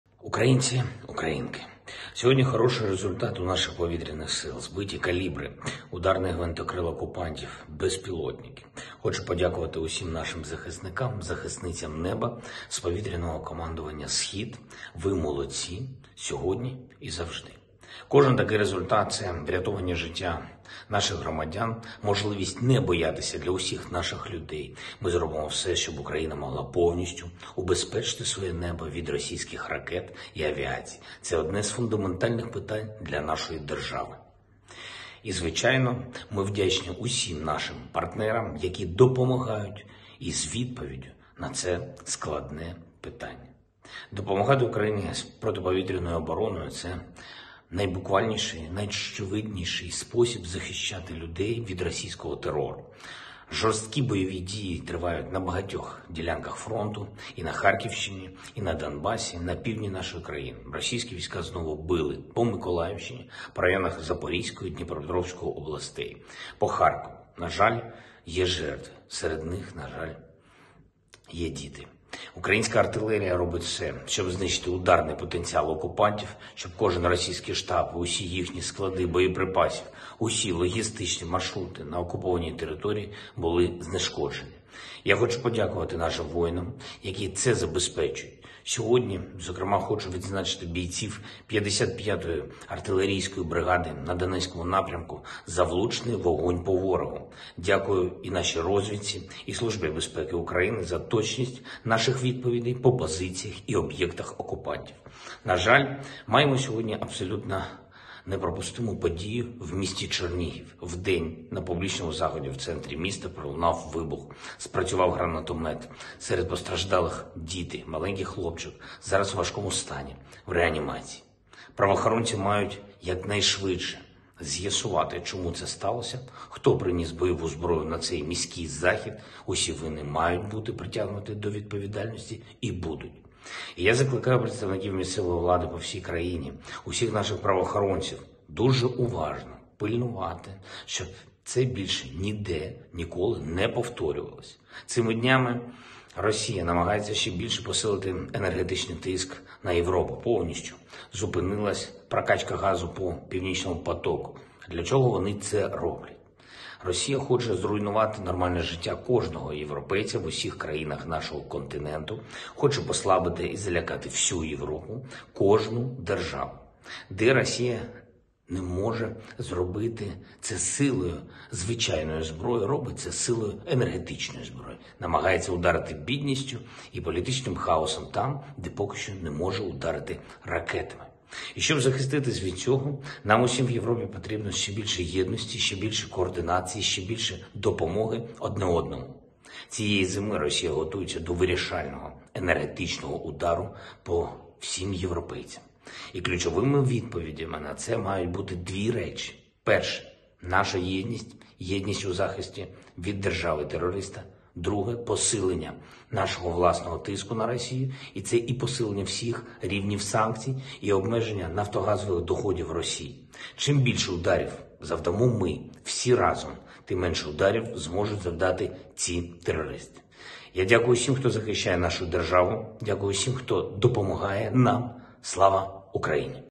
💥 Звернення Зеленського за 3 вересня
У вечірньому зверненні від 3 вересня Володимир Зеленський підняв тему енергетичного шантажу росії та вибухи у Чернігові.

Ukraine.Media
Lviv.Media